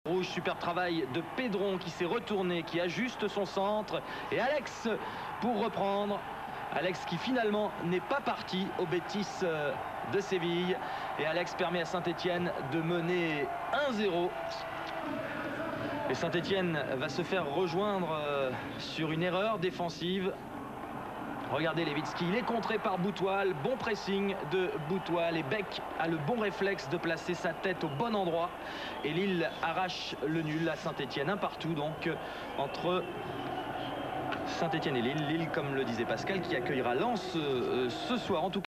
ASSE 1-1 Lille - 8e journée de L1 2000-2001
Un dégagement hasardeux de Maxim Levytsky coûte la victoire à des Verts qui avaient ouvert la marque par Alex. Ils sont 11es.

Une défense bien friable en ce ce début de saison...

Images TF1